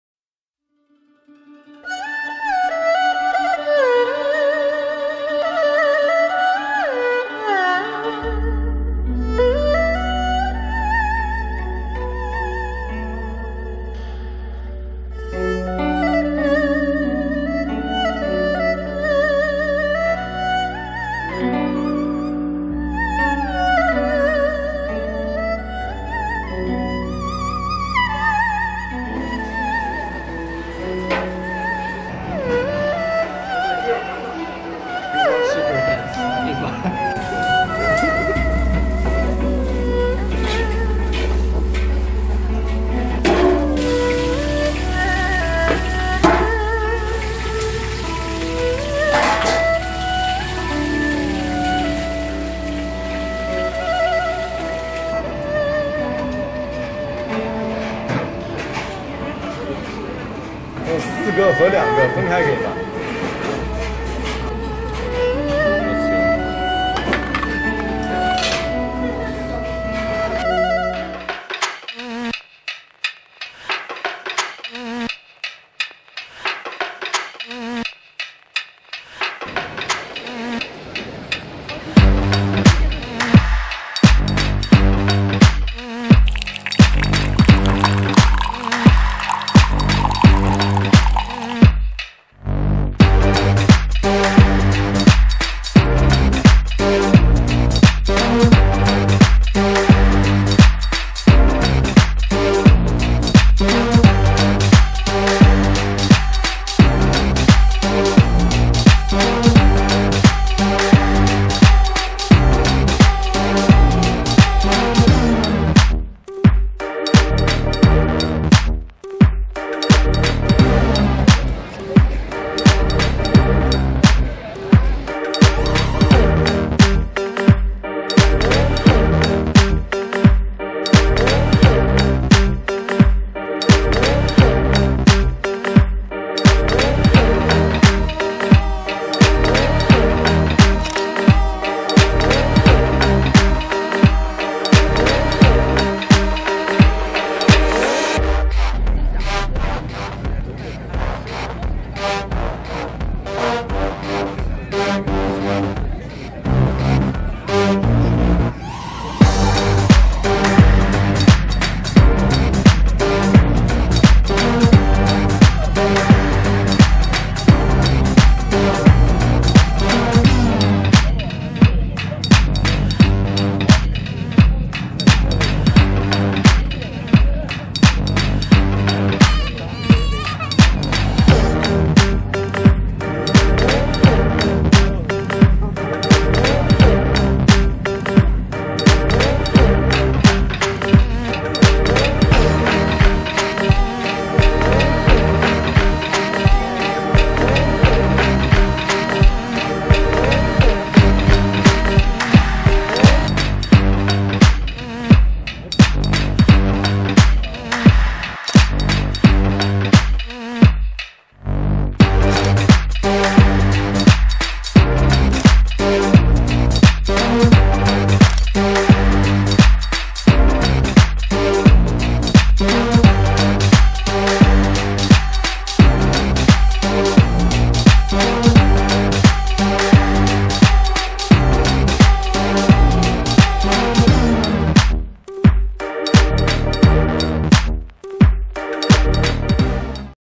Shanghai by the night
When you have only one night to explore this amazing city all you do is eat, eat and eat as much you can.